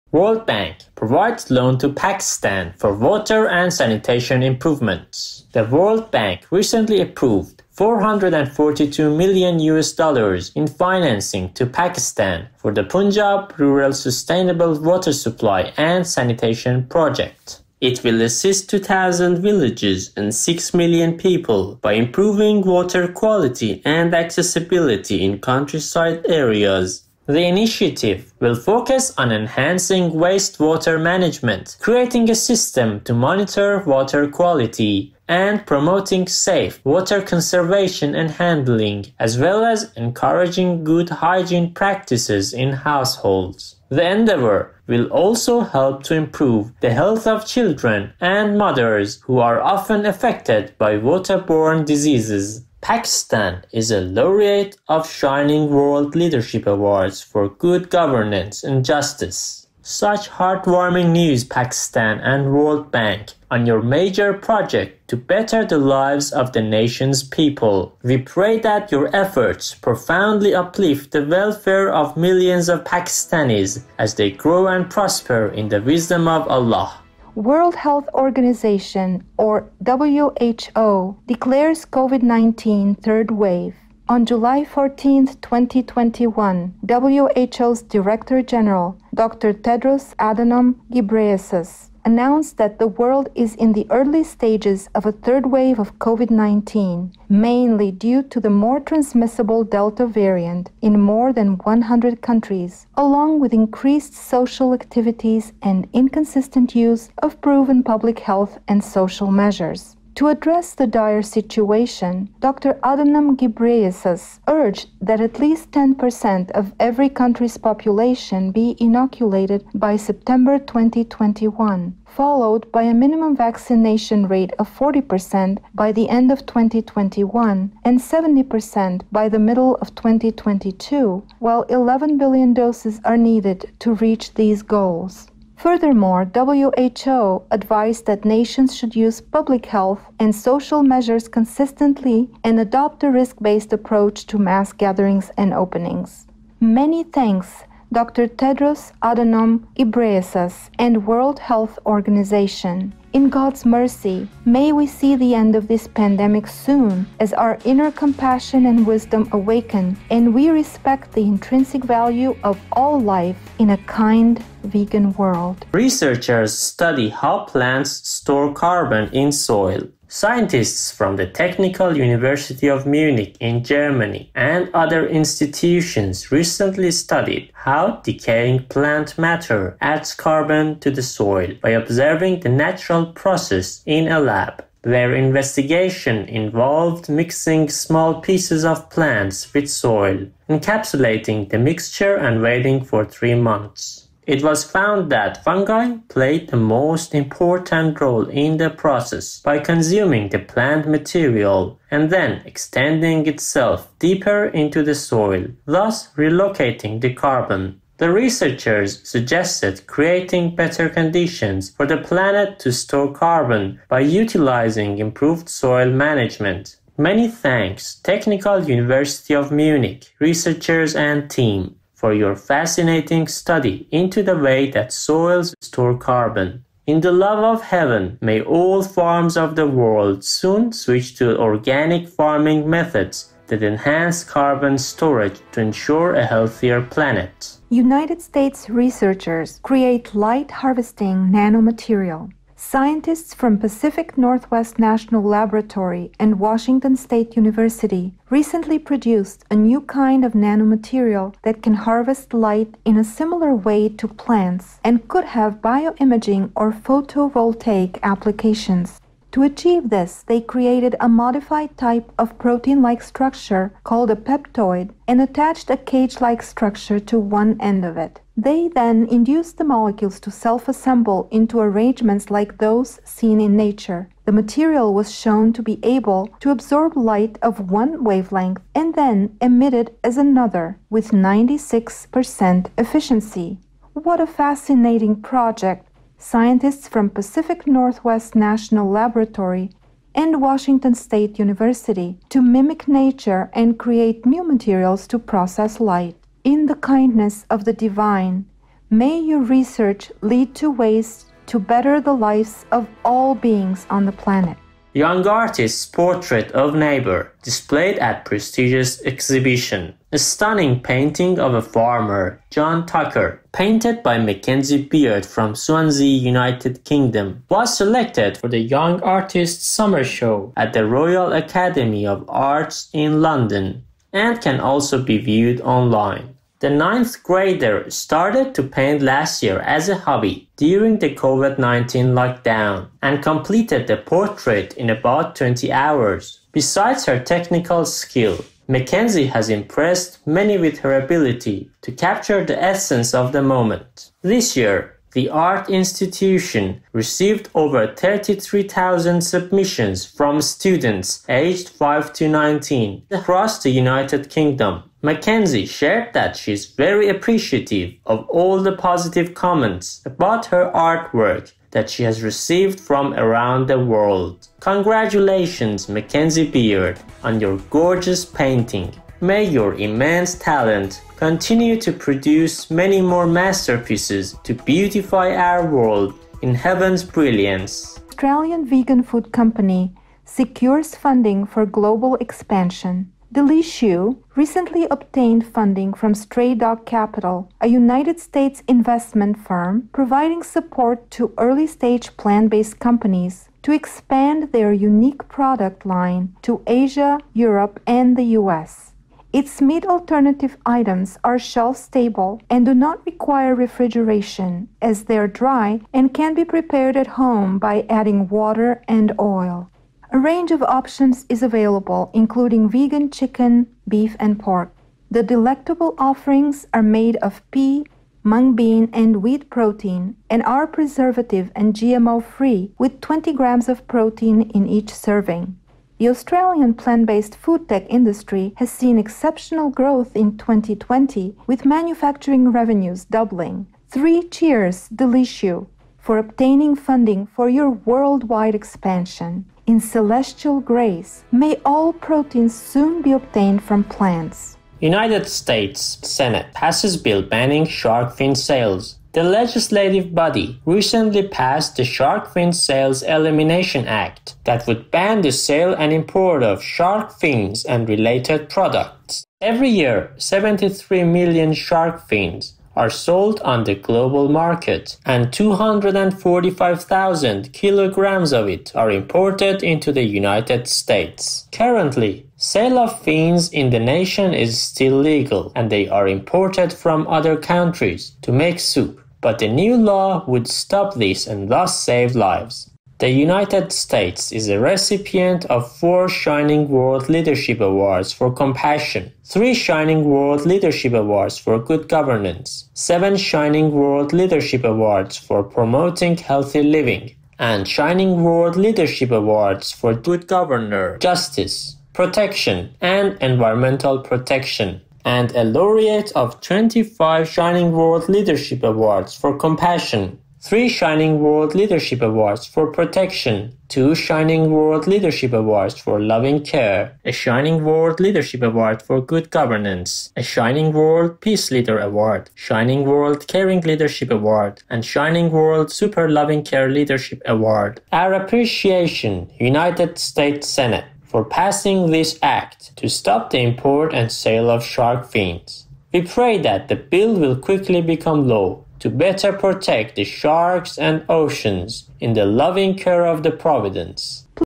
#1409 Watch Good News to Learn English 看新聞學英語 20210812
In today’s news,
今日新聞提要：

Pakistan and World Bank to improve sanitation and water access for millions of people,
巴基斯坦和世界銀行為數百萬人民改善衛生和供水設施；

World Health Organization warns of COVID-19 third wave’s start,
世界衛生組織示警新冠肺炎疫情第三波開始；

researchers examine how decaying plants store carbon in soil,
研究人員檢測出腐化植物如何儲存碳於土壤；

scientists develop light-harvesting nano-material in United States,
美國科學家開發出可收集光能的奈米材料；

talented teen artist’s painting displayed at Royal Academy of Arts in United Kingdom,
才華出眾的青少年藝術家畫作展出於英國皇家藝術研究院；

vegan food company from Australia goes global,
澳洲純素食品公司產品擴展至全球；

United States Senate passes bill to protect sharks from shark finning.
美國參議院通過了禁止販售鯊魚魚翅的保護鯊魚法案。